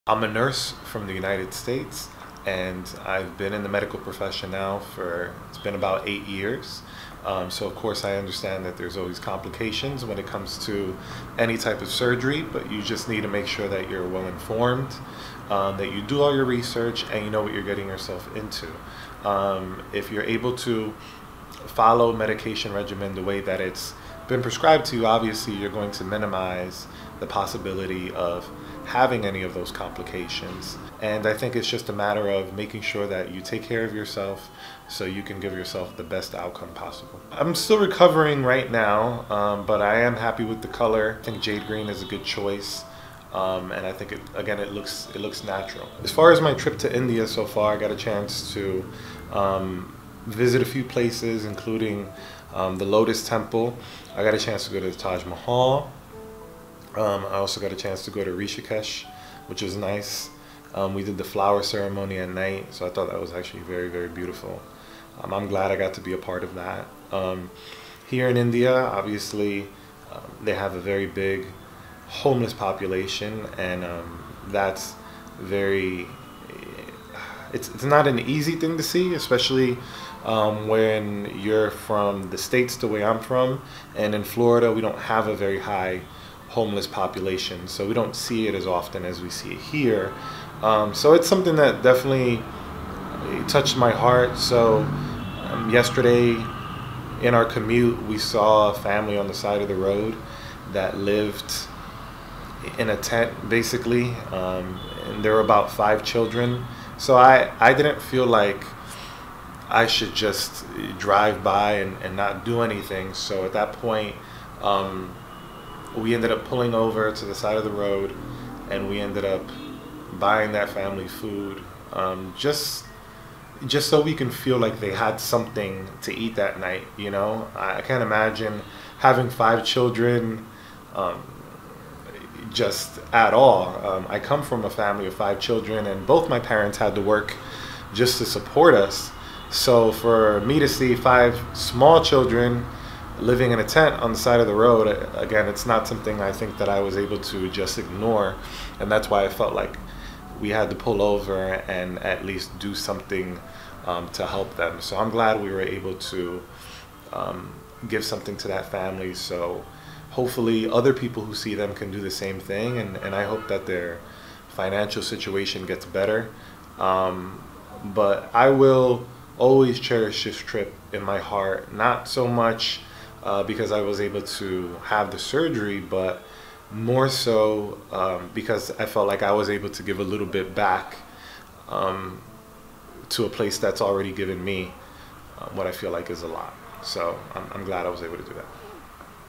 Nurse has an eye color change procedure - Brightocular
Nurse has a cosmetic surgery to change eye color. Jade Green colour